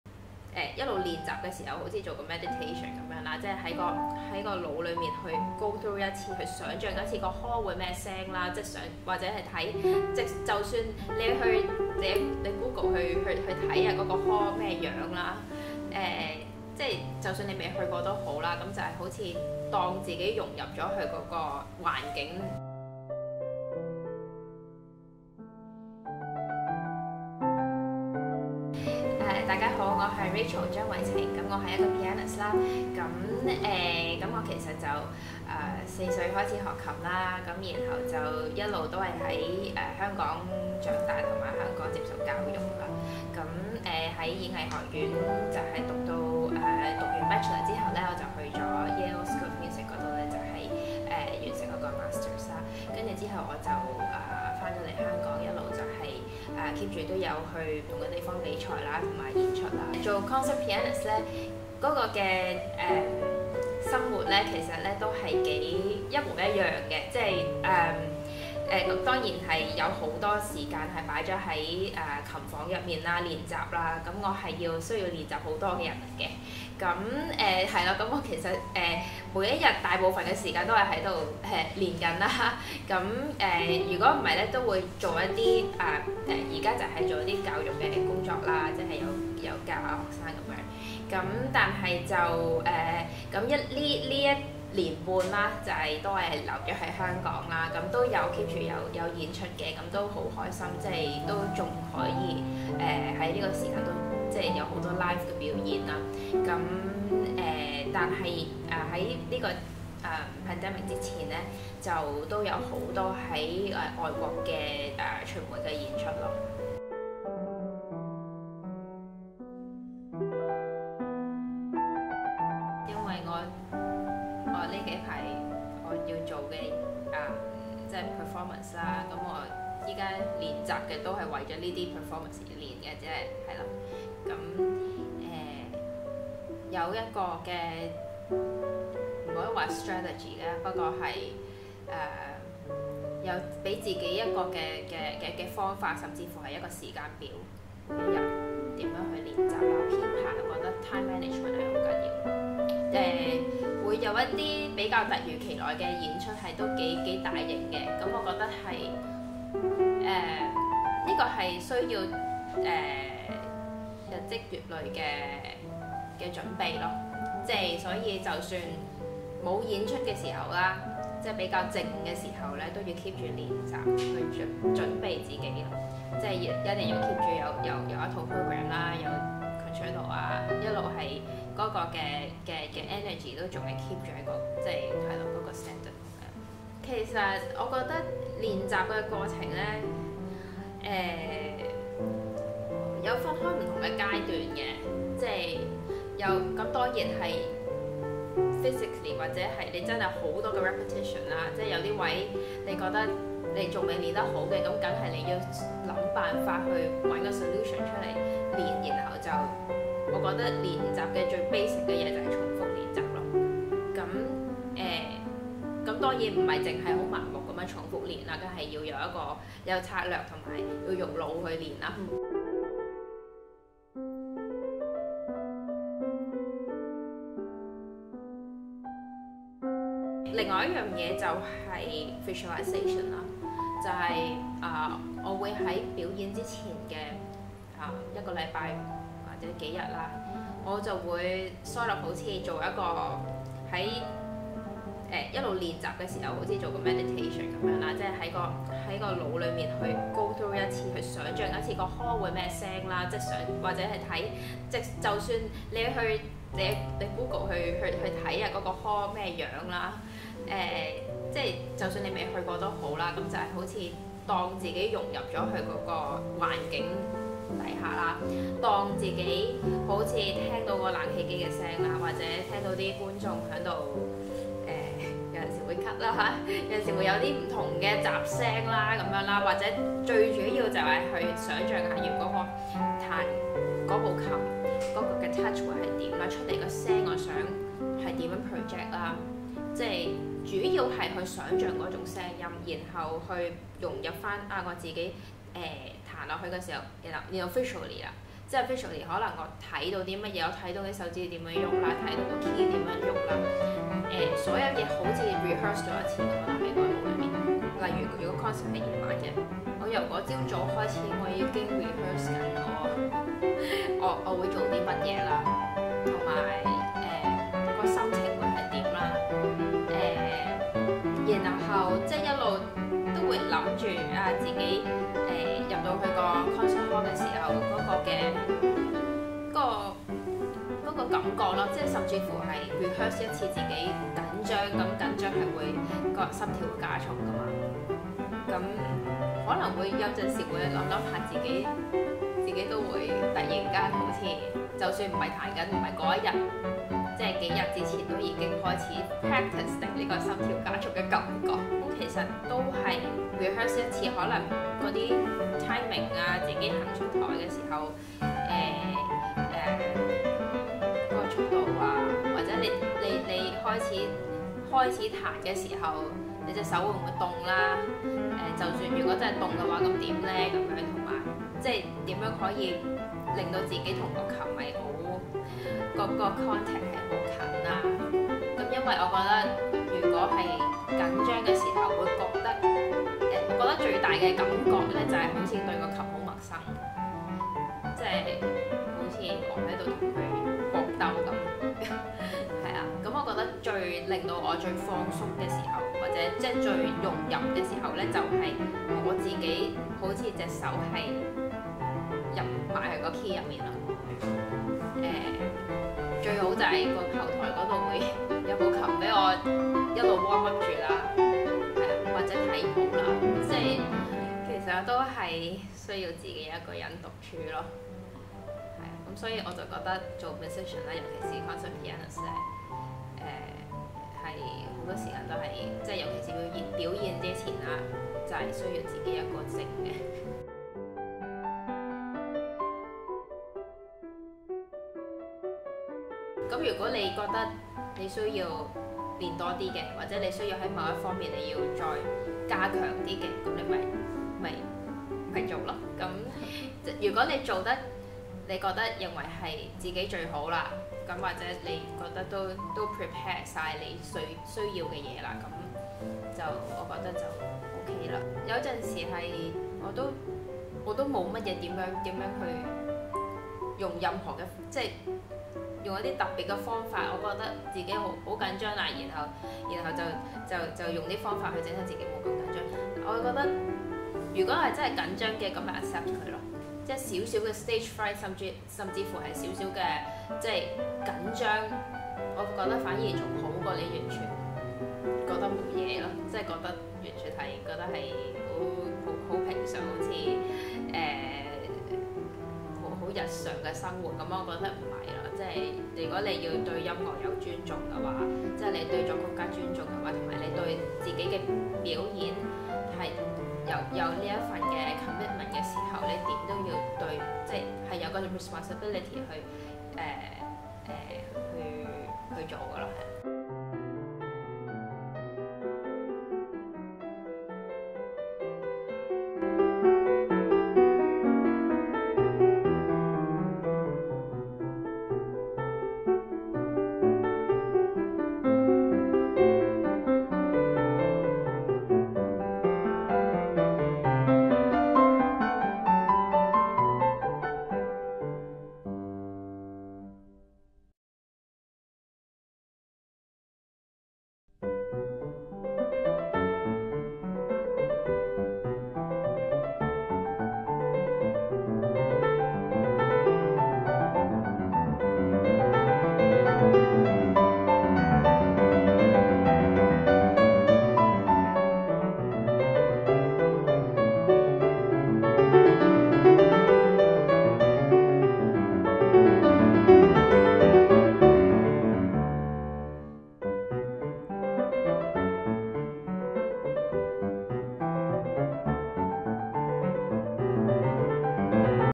[EP3] 鋼琴家張緯晴小姐：排練時，想像親臨演出現場的感覺 [巔峰表現專訪：上篇]
鋼琴家張緯晴小姐分享她在 現場演奏 前，融合 靜觀 技巧的排練方法。
Pianist Rachel Cheung shares her secret mindfulness visualisation visualization techniques as she prepares for LivePerformance

2:01恆常練習，應付突發表演。
Practise consistently, and stay always ready for performance.

3:44 透過默想，預演搭上臺板時的細微身心反應。
Rehearse by visualising every tiny physical and psychological responses.

7:10 若已做好準備，接納緊張情緒。
It’s okay to be nervous. One is nervous because one cares.

訪問日期：2021年9月23日
Interview Date: 23 September 2021